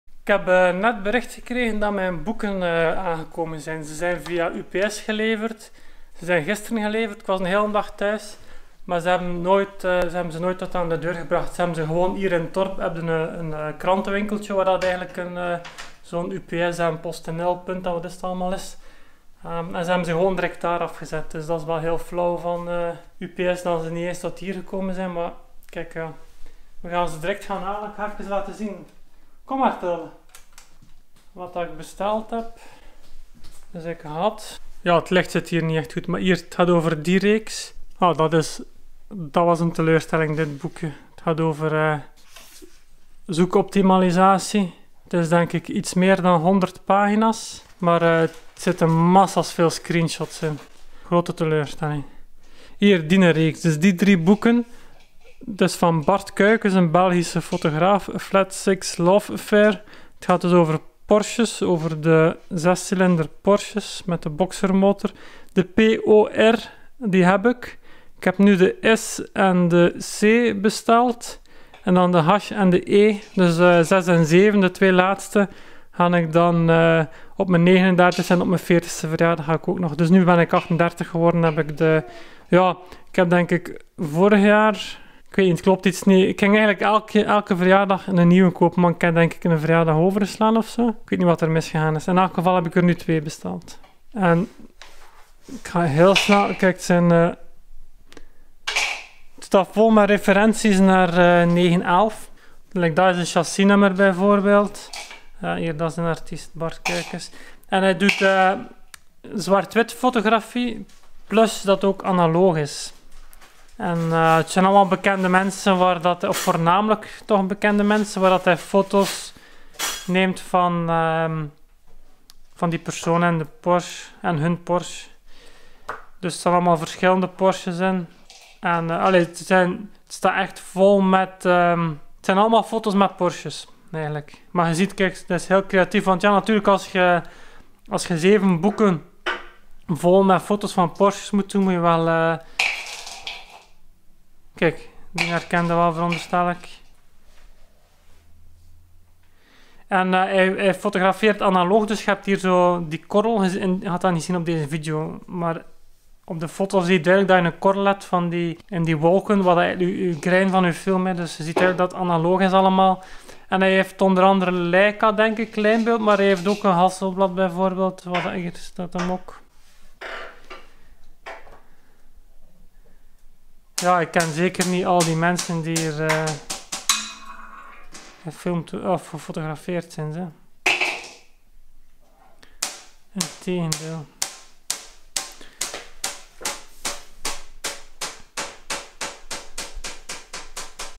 Porsche boeken set "a Flat 6 Love Affair" van Bart Kuykens bijna compleet
En snelle kijk in een van de fotografie boeken van Bart Kuykens. Bart is een Belgische fotograaf die met analogie fotografie prachtige reeksen maakt rond de 911 Porsche modellen. Vaak ook van beroemdheden over heel de wereld.